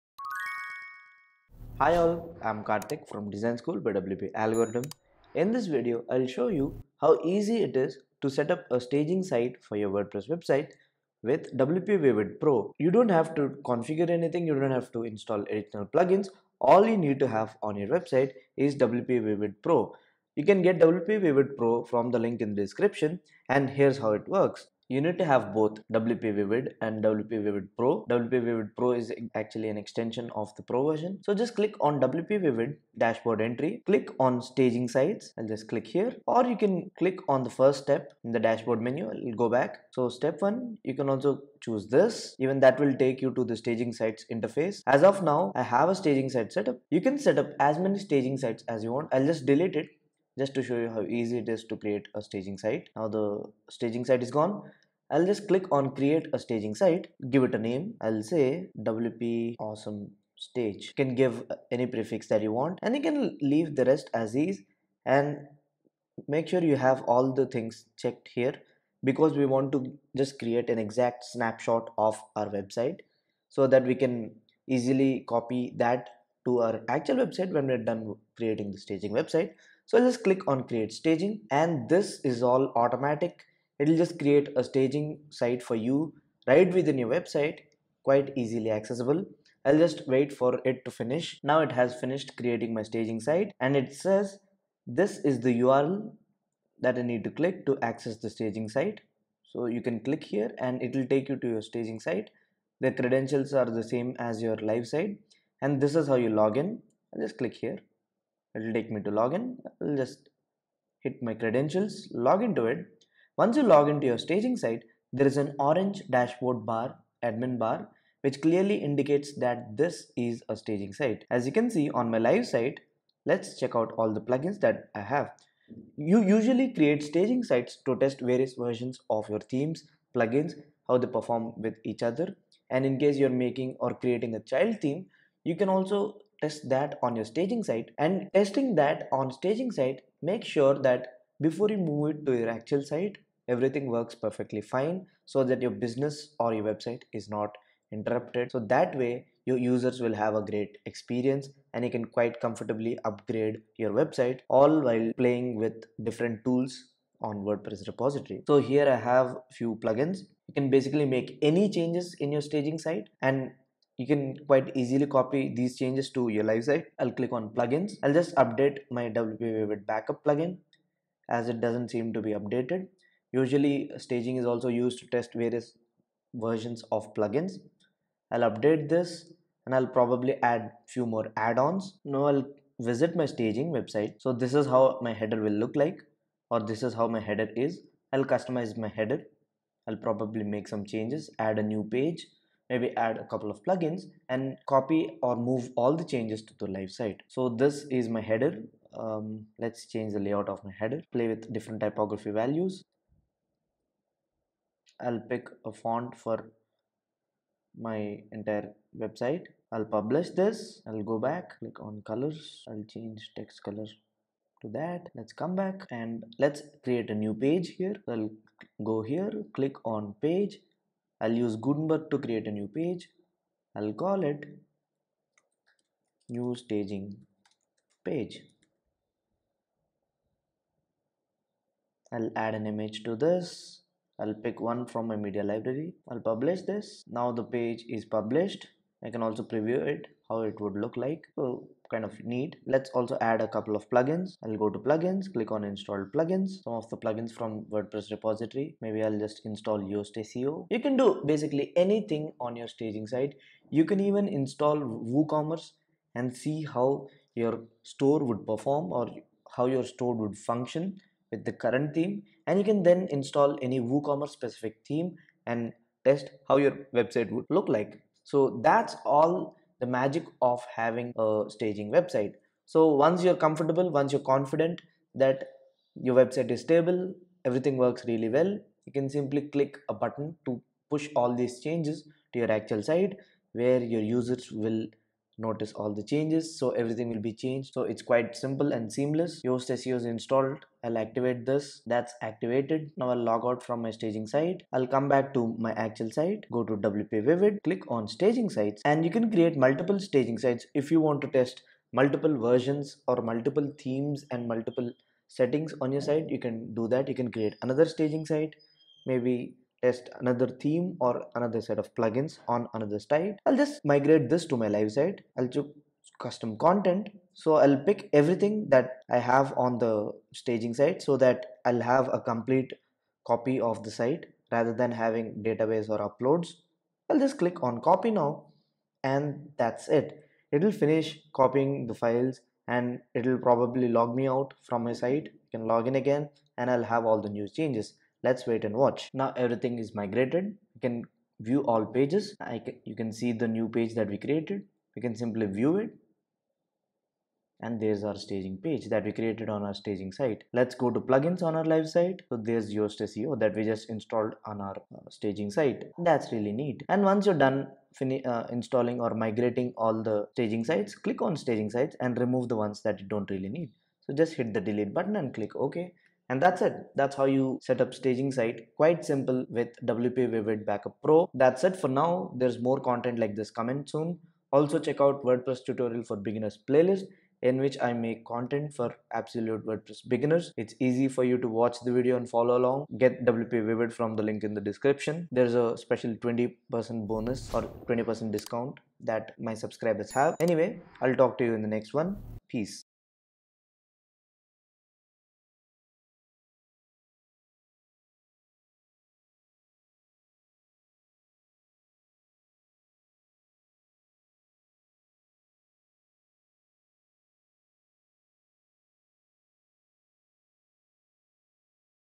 WordPress staging setup and copy to live site - with WpVivid pro plugin
Learn how to setup WordPress staging site to test out various themes, plugins and configurations by using Wpvivid pro plugin.

Even better copy staging site to live server with just click of a button. Any site, any hosting, any configuration, easily configurable.